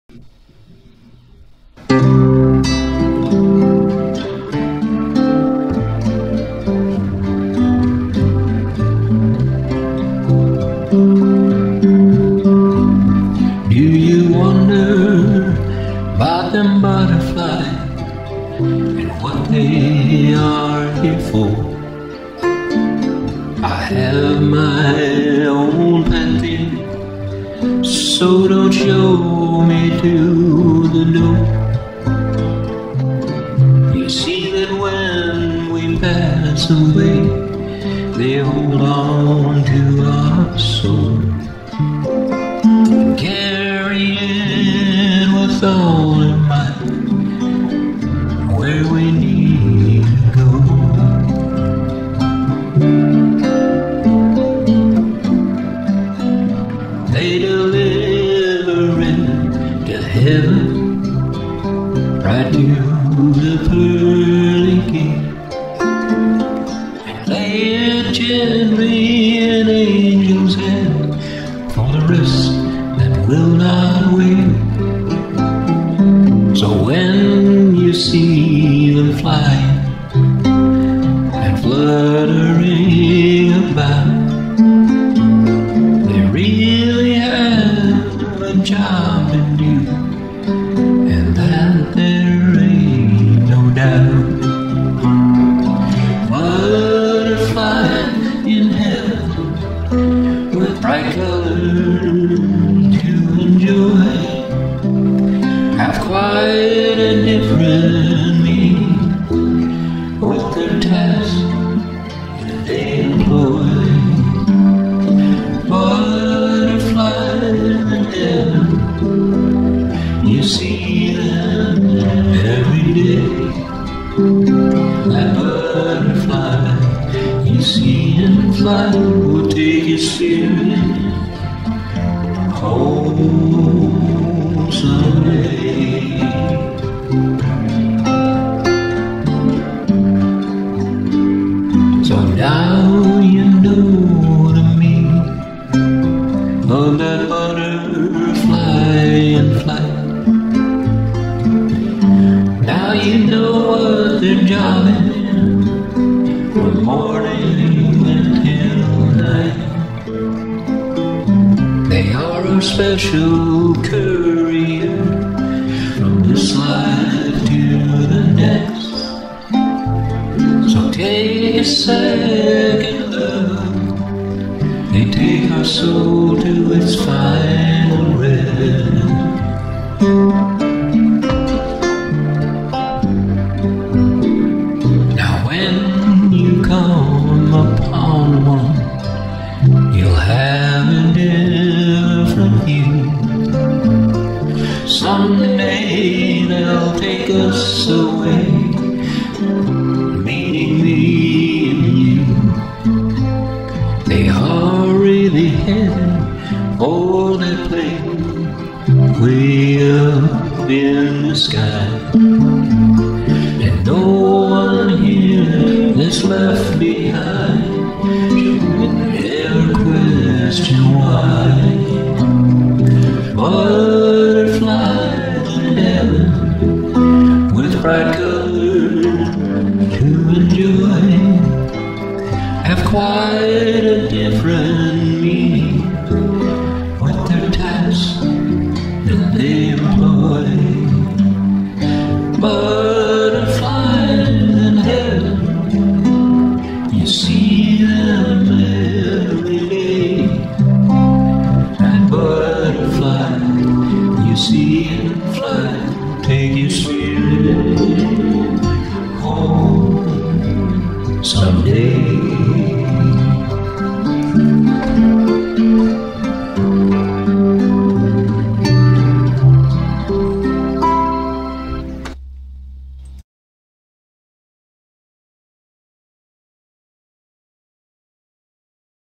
butterflys in Heaven
My vision of what butterflies are really here for. Dedication to all who have lost someone and where they go.